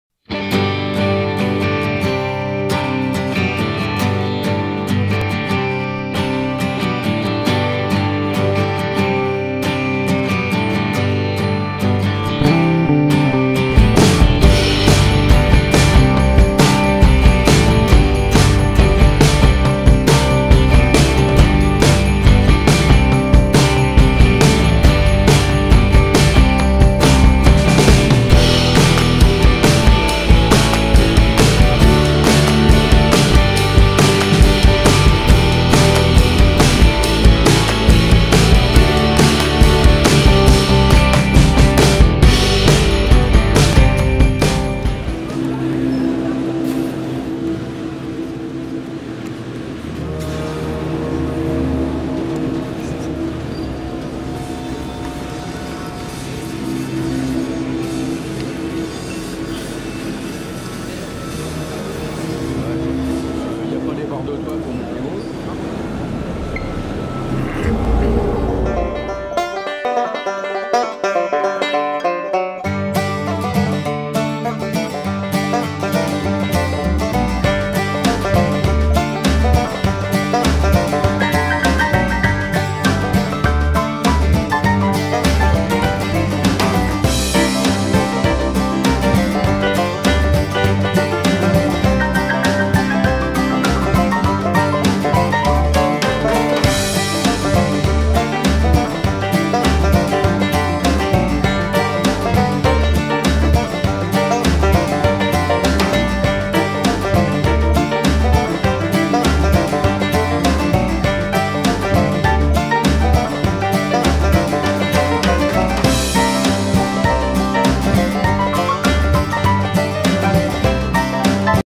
PORSCHE SALON DU MONDIALE 2016
VOICI DES JOLIES PORSCHE
TOUTES EXCEPTIONNEL IBRID COMME
ELECTRIQUE ET THERMIQUE